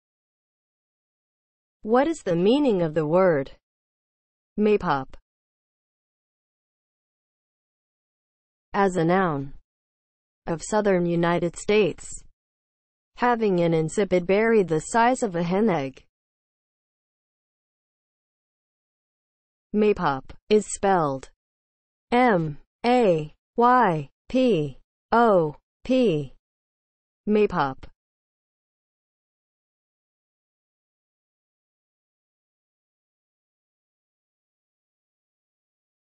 What is the meaning of the word MAYPOP?
Definition and spelling of the word MAYPOP.

(00:00) Intro
(00:07) As a noun
(00:18) Spelling

As a noun (00:07):
• of southern United States; having an insipid berry the size of a hen egg